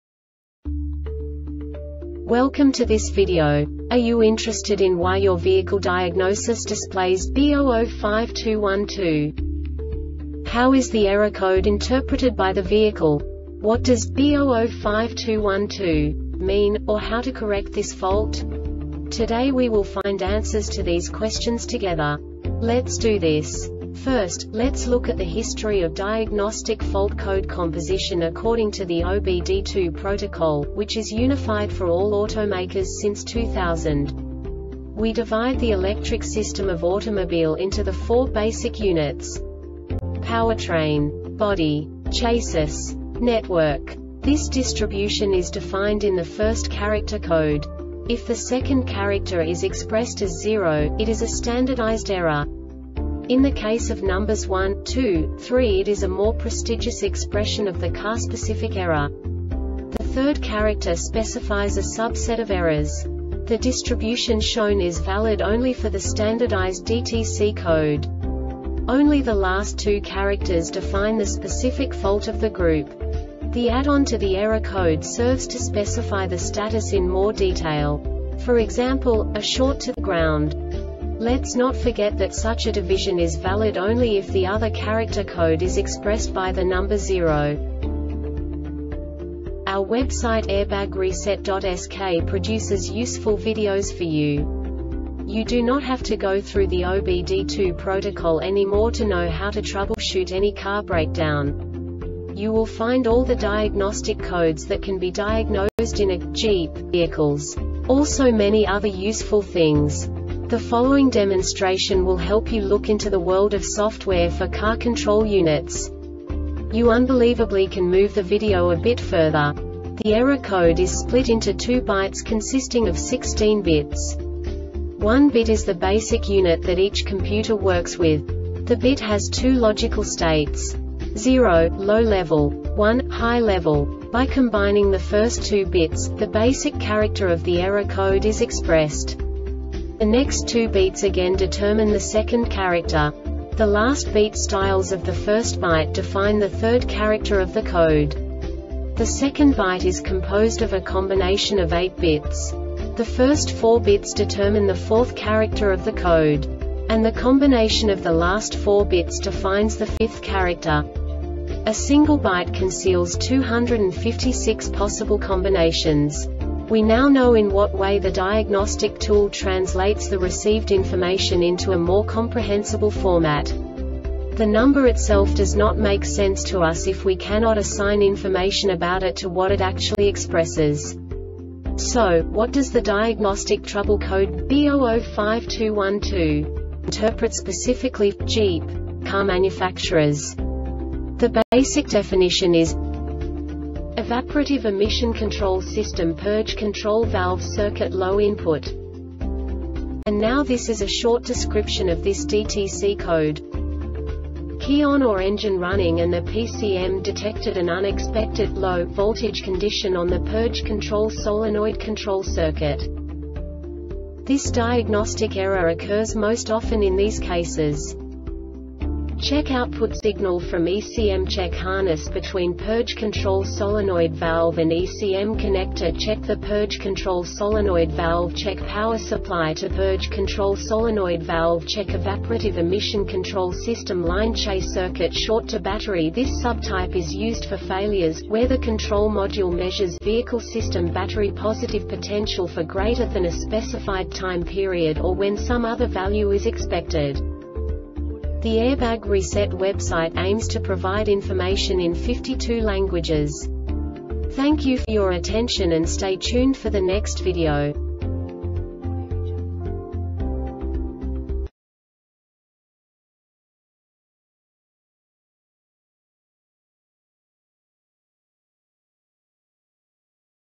DTC Jeep B0052-12 Short Explanation
Contents:
0:21 Basic DTC analysis according to OBD2 protocol standard.
1:48 Insight into programming
2:58 A diagnostically understandable description of the Diagnostic Trouble Code
3:05 Basic error definition

Make:
Jeep
Code:
B0052 12
Definition:
Evaporative Emission Control System Purge Control Valve Circuit Low Input
Description:
Key on or engine running;and the PCM detected an unexpected "low" voltage condition on the Purge Control solenoid control circuit.
Cause:
Check output signal from ECMCheck harness between purge control solenoid valve and ECM connectorCheck the purge control solenoid valveCheck power supply to purge control solenoid valveCheck evaporative emission control system lineChe
Failure Type: